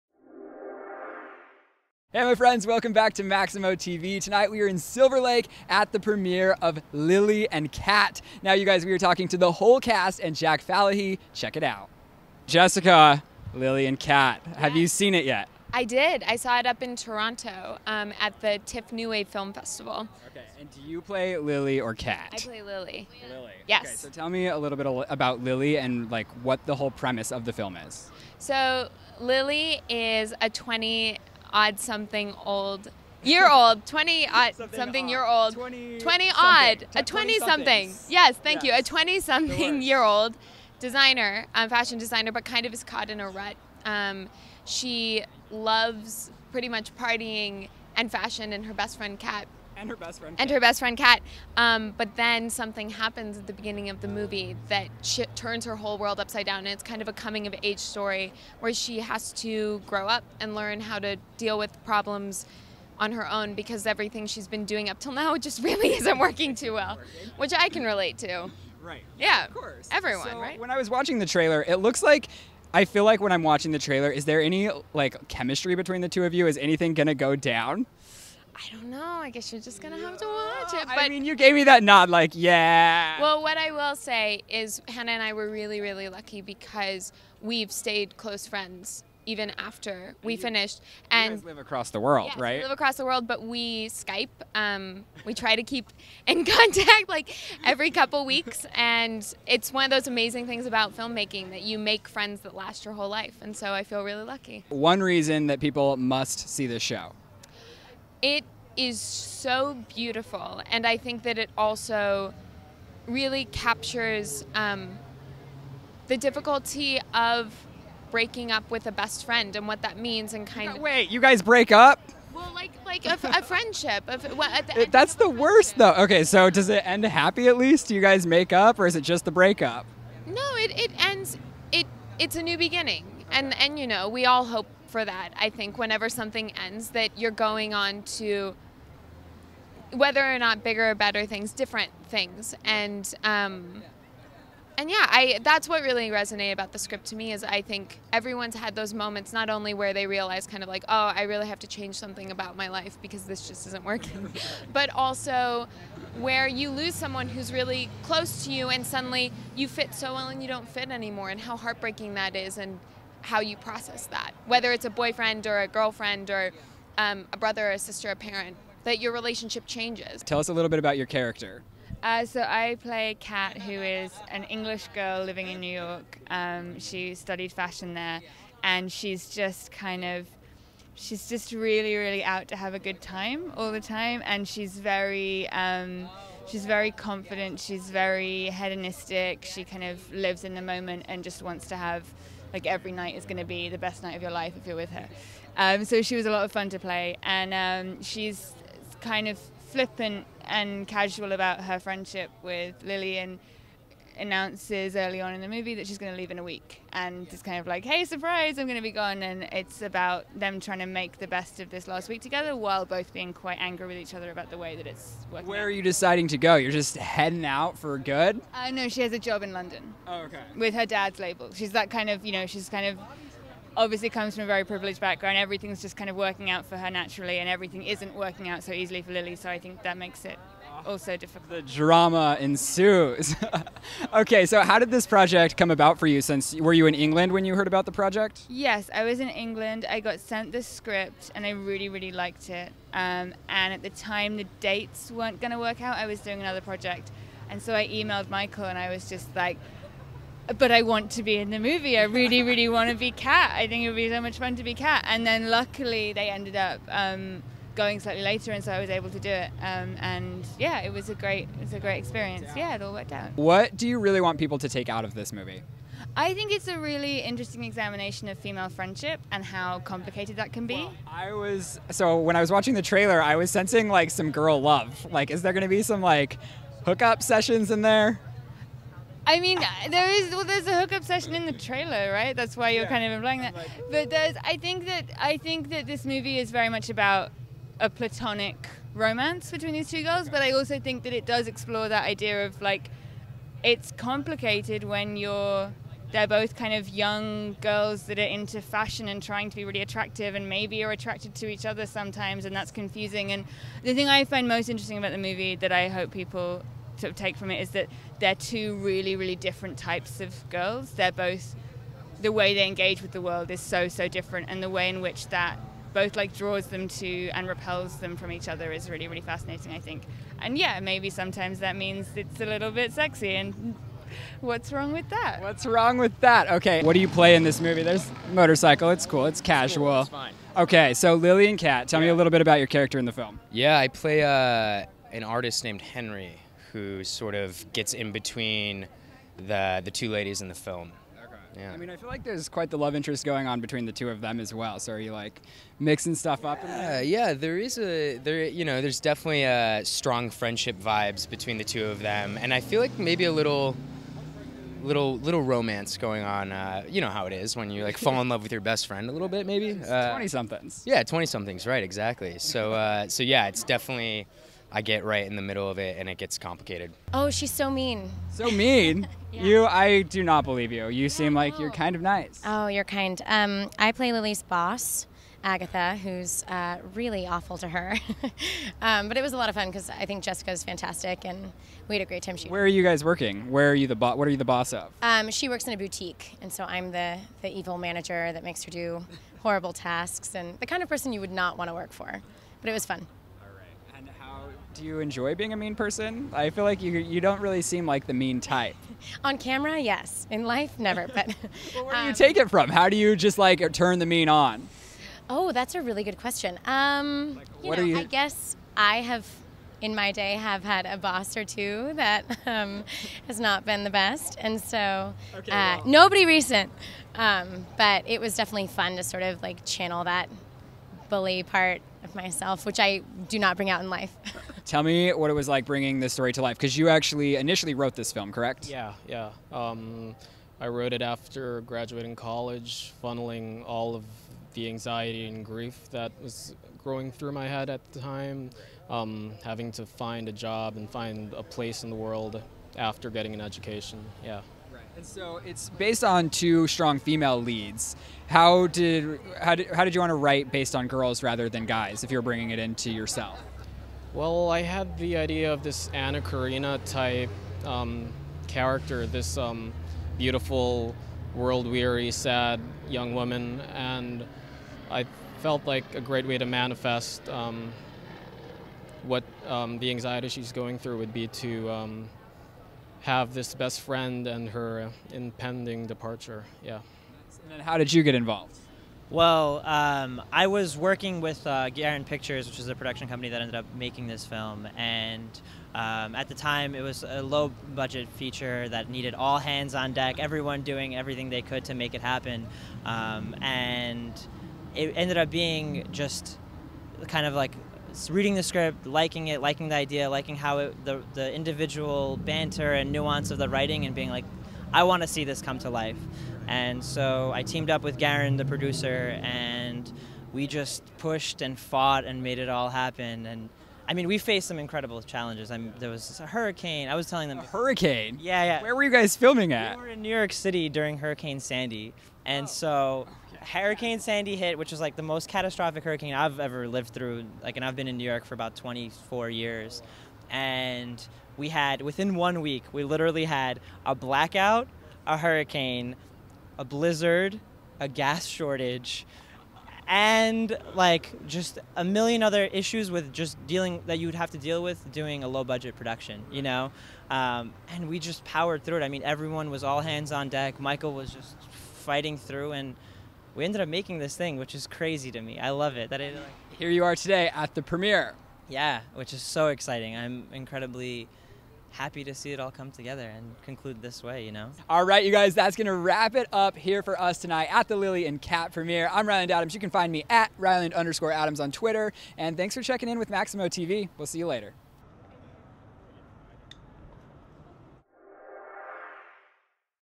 Hannah Murray, Jessica Rothe, Jack Falahee "Lily & Kat" Los Angeles Premiere
Exclusive red carpet interviews with Hannah Murray, Jessica Rothe, Jack Falahee, Mimi Gianopulos, Director Micael Preysler and Producer Areg Barsegian at LILY & KAT premiere at Vista Theater in Los Angeles, Ca March 21st, 2015 - Interviews by Ryland Adams (@Ryland_Adams)

Set in New York City, the film follows a naive fashion school graduate named Lily (Jessica Rothe) who finds her world turned upside down when her reckless best friend Kat (Hannah Murray) announces she’s moving away to London in a matter of days. At a Lower East Side art opening the next night, they meet the enigmatic rising artist Henri (Jack Falahee), who Lily quickly takes a liking for. With less than seven days left and a new attractor between them, Lily and Kat will find their “unbreakable” friendship put to the test.

Jessica Rothe as Lily, Hannah Murray as Kat, Jack Falahee as Henri, Mimi Gianopulos as Agatha